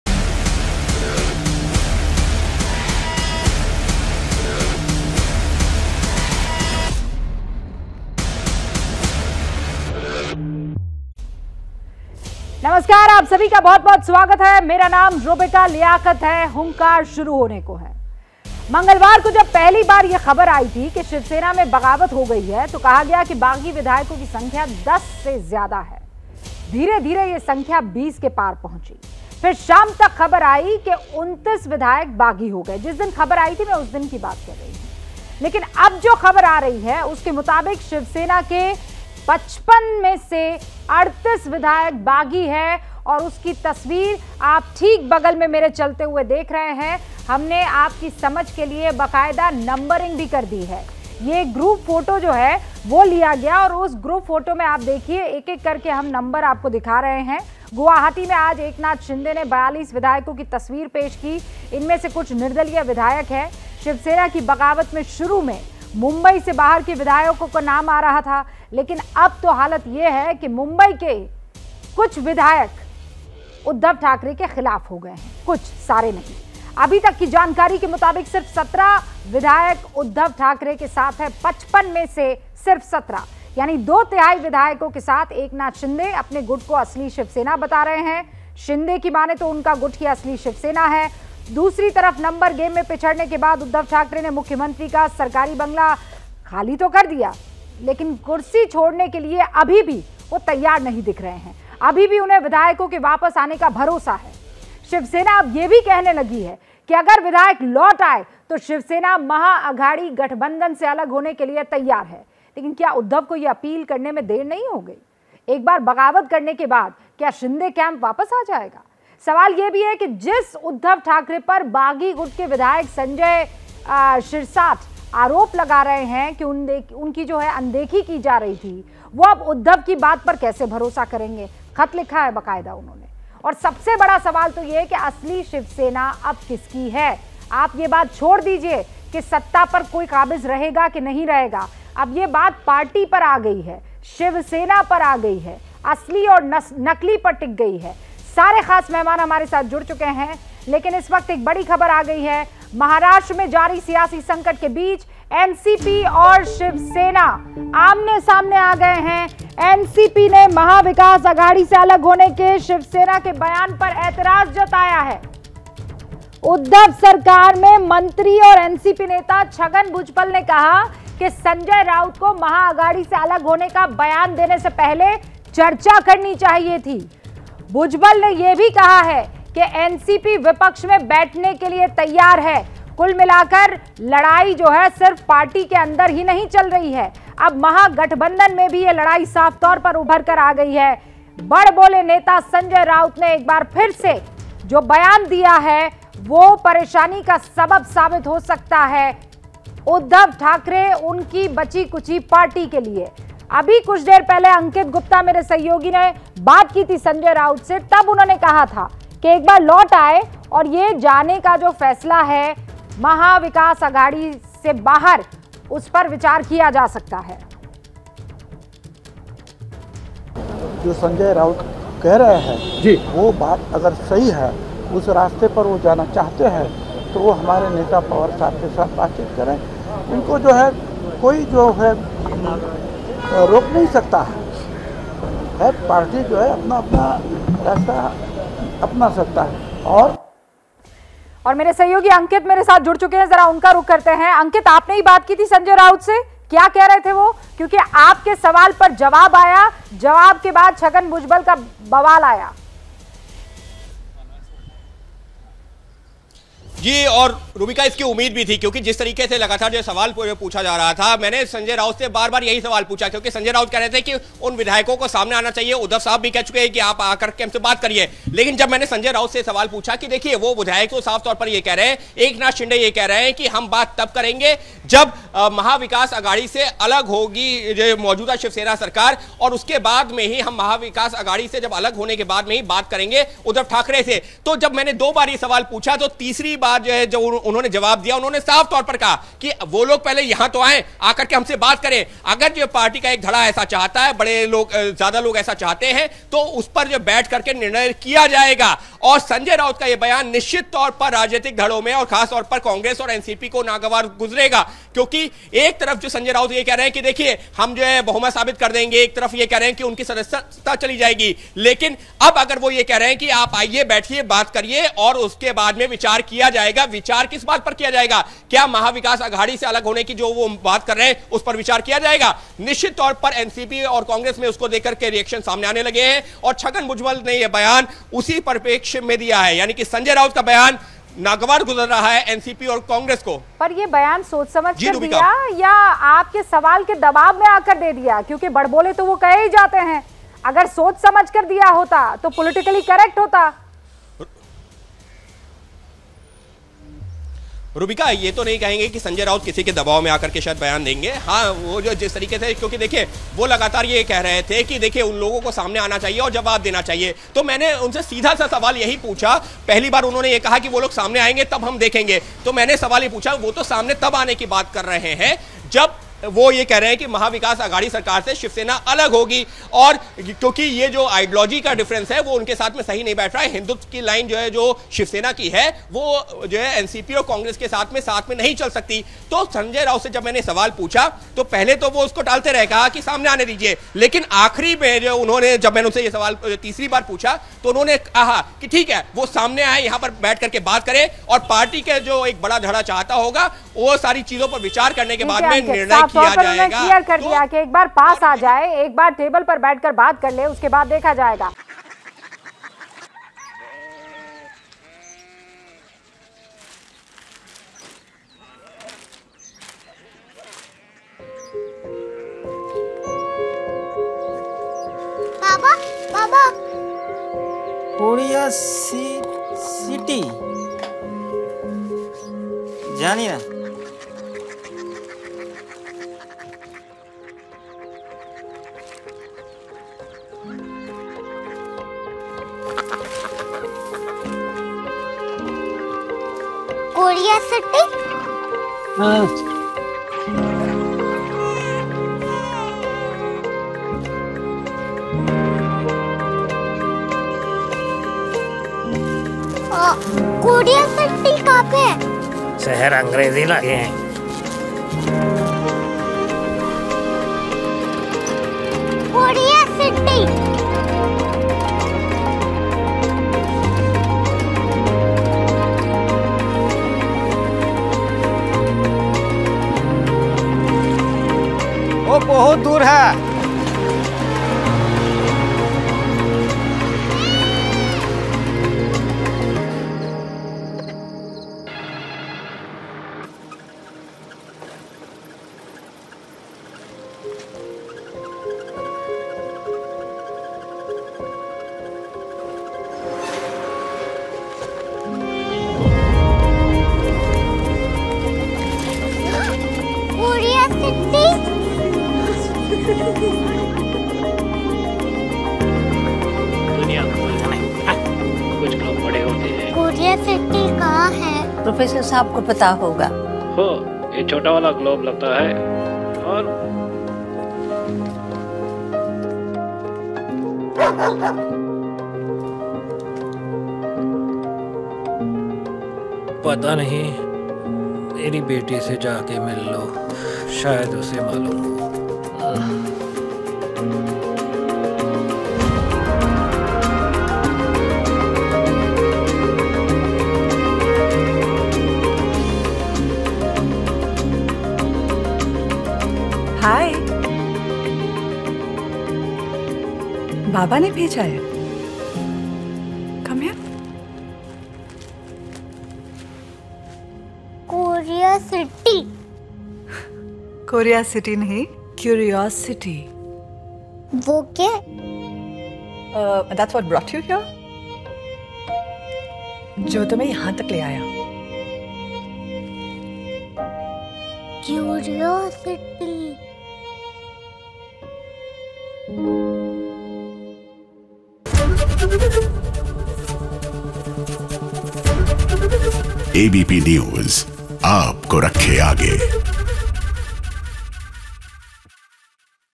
क्या अब Eknath Shinde की हो जाएगी Shivsena ?
मंगलवार को जब पहली बार ये ख़बर आई थी कि शिवसेना में बग़ावत हो गई है तो कहा गया कि बाग़ी विधायकों की संख्या 10 से ज़्यादा है...धीरे-धीरे ये संख्या 20 के पार पहुंच गई...फिर शाम तक ख़बर आई कि 29 विधायक बाग़ी है...